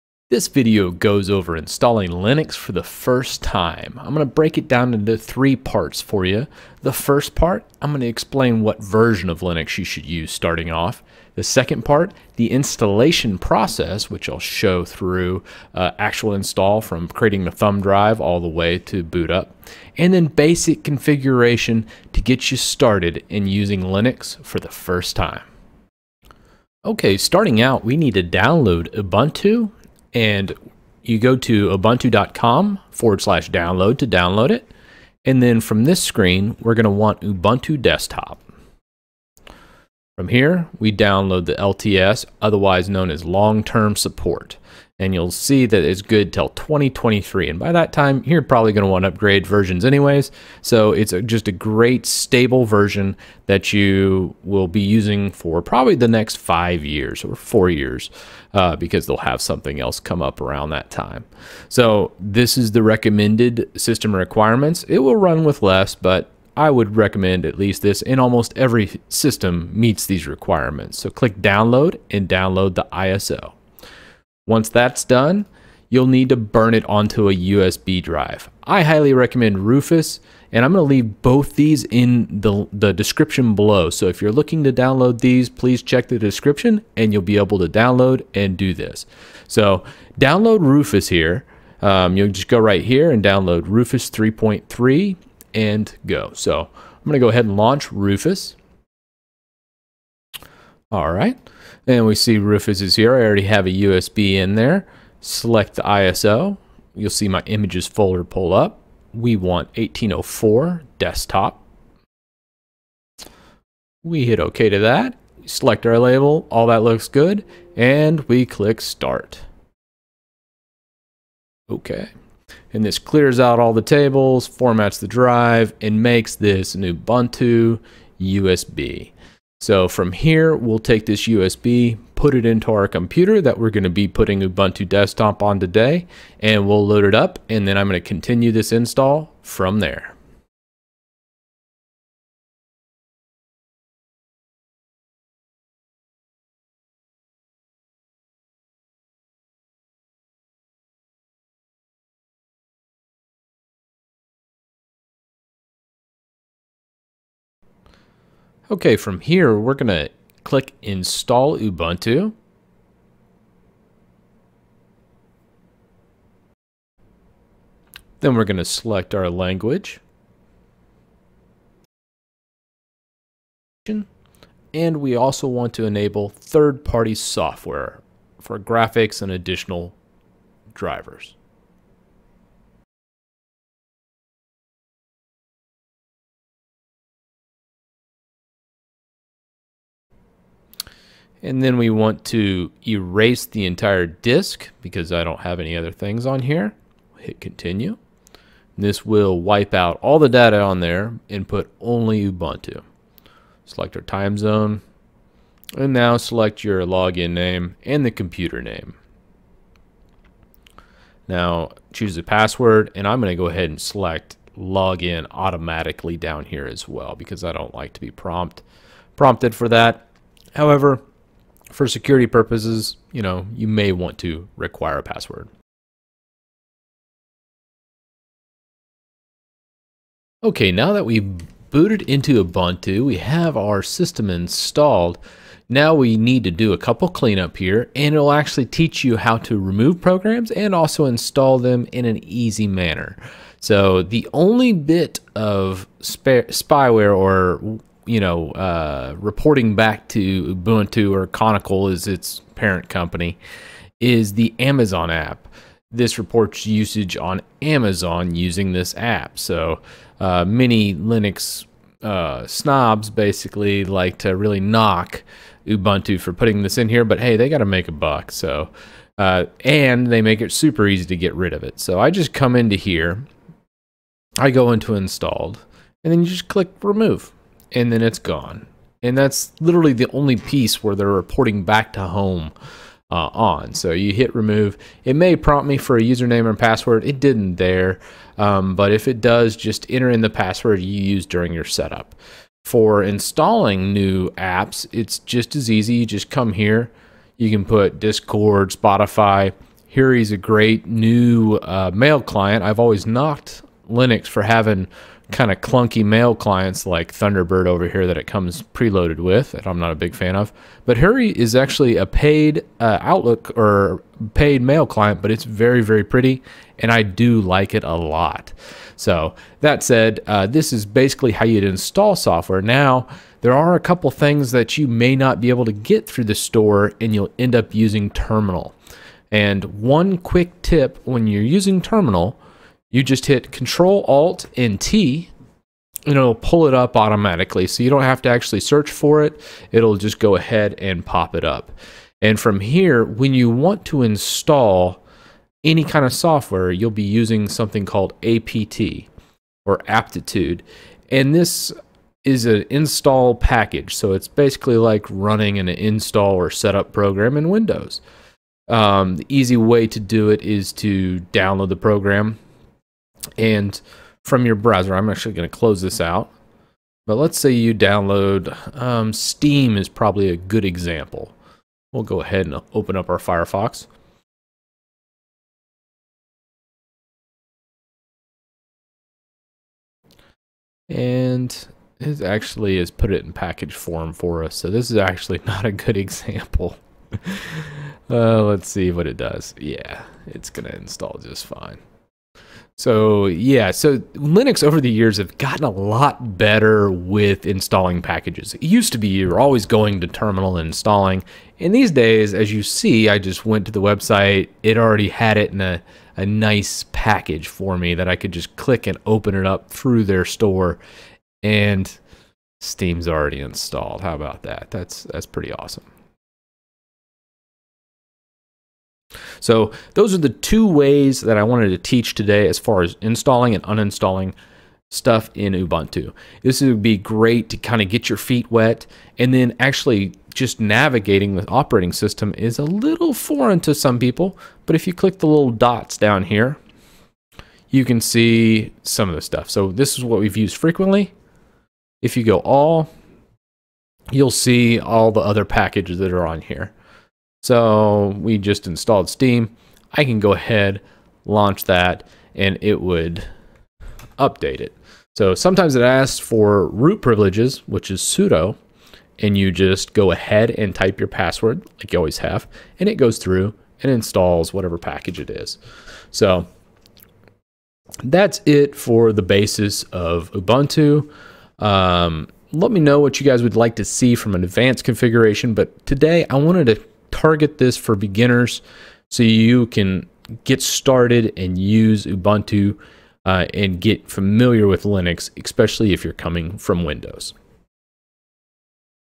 How to Install Linux for Beginners
This video goes over installing Linux for the first time. I will break it down into 3 parts:
1. Choosing which version of Linux
2. Installation Process

3. Basic configuration

Choosing the right version or flavor of Linux
-Latest Ubuntu Desktop LTS

Installation Process
- Downlaod ISO for ubuntu
- Create a USB Thumb drive
- Install

Basic Configuration
-Installing Software .